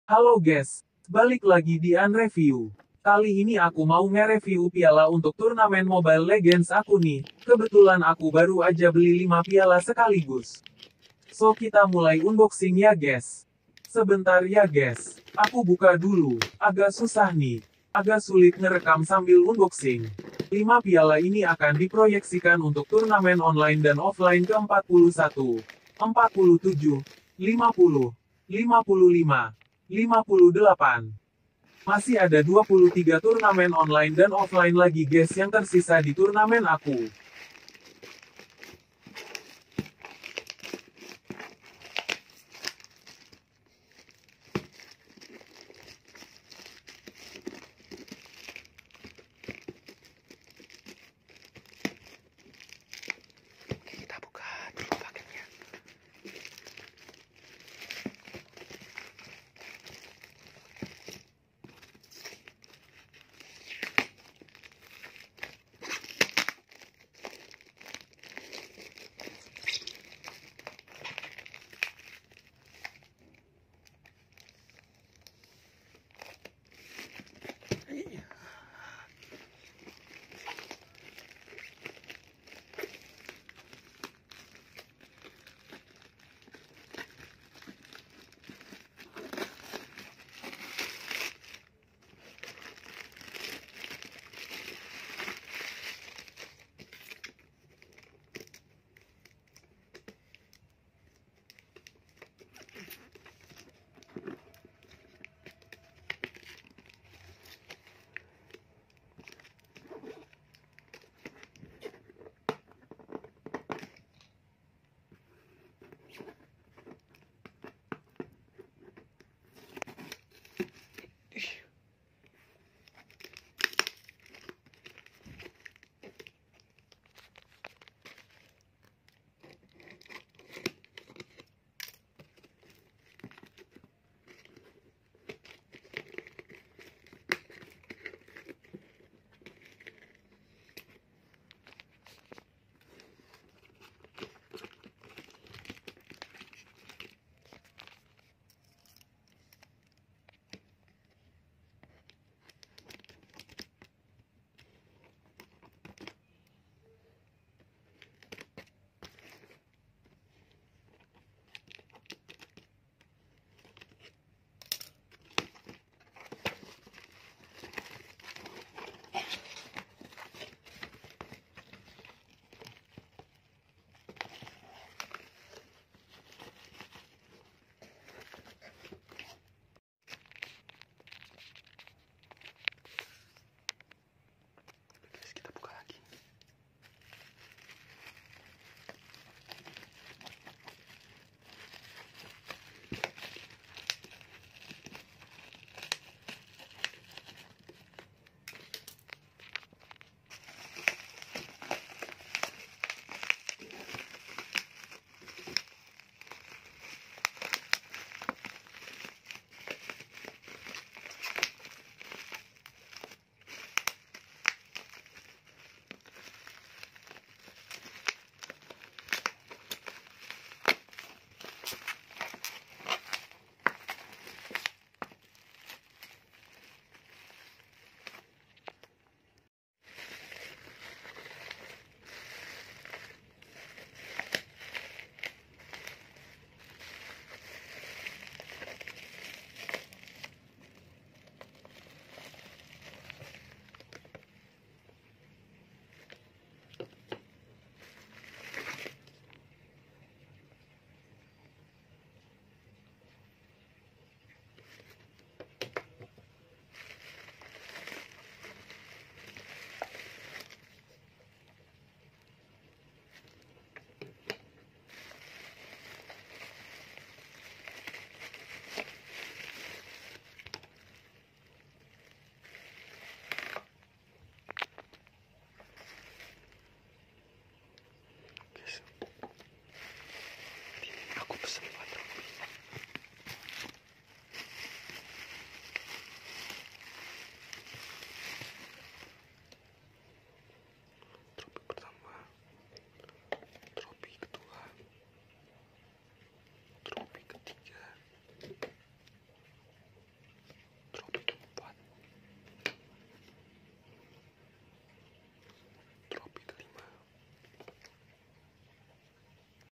ReviewAn (Part 2) | Unboxing Piala Akrilik Turnamen Mobile Legends Untuk Event Organizer Aku 😊
Hello Genks. Di Video Kali Ini Aku Mau Unboxing Paket Yang Baru Aja Datang Yaitu Piala Akrilik Untuk Event Organizer Aku